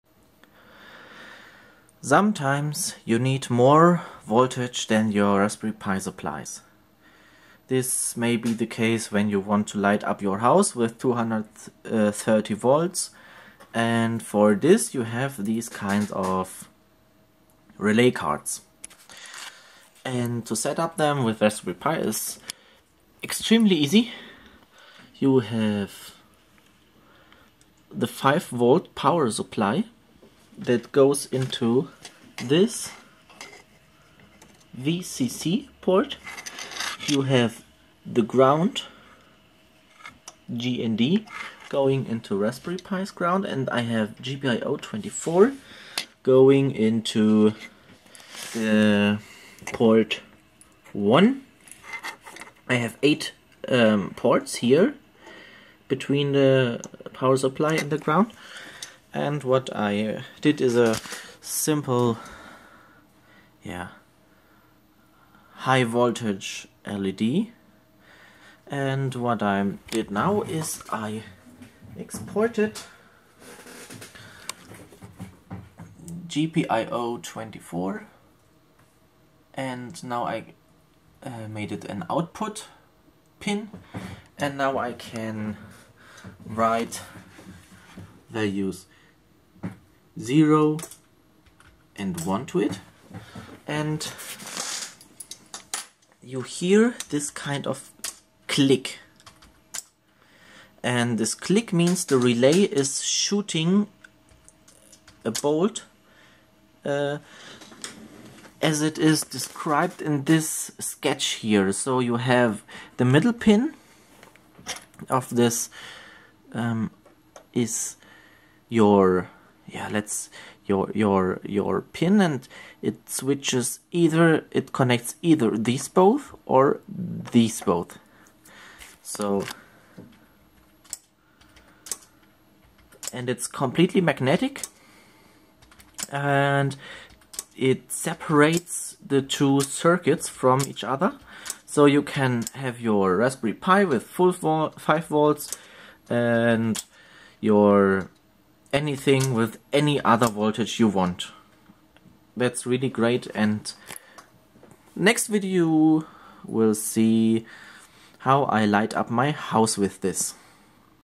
Raspberry Pi and Relais Card
Connecting relais cards to raspberry pi is rather easy: You connect VCC to 5v, GND to GND and GPIOs to input. Then you export your GPIO, set direction to "out" and write values to it.